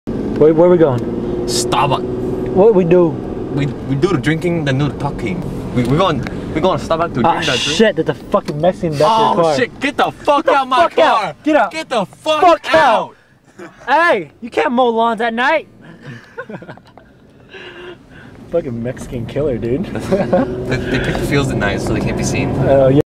Mexican Killer
Brett and I are driving to Starbucks one night, then all of a sudden this mexican killer appears in the backseat.